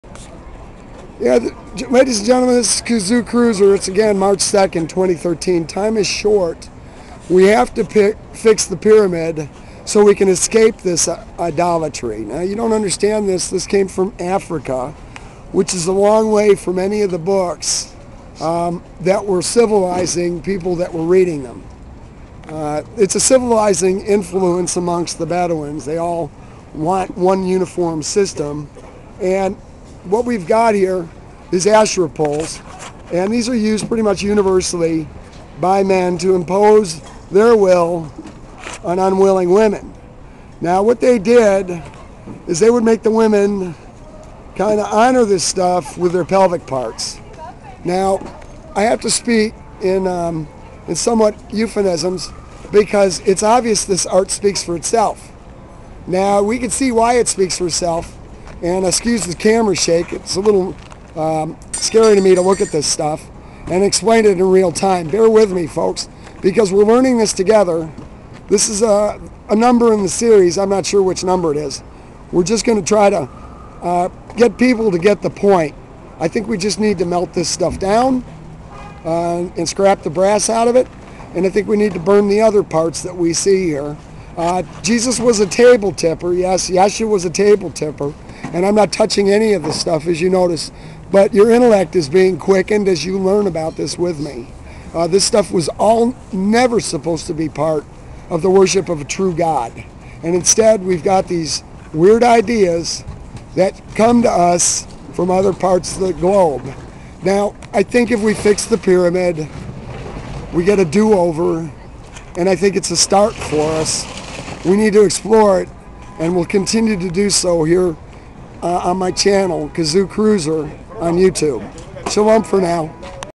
Asherae_04
Another in the series of ignorant idolatry.  The shame is that the uninitiated do not see the dark side of this malice.  All they see is "pretty art" made by ancient craftsmen.  It all needs to be melted down.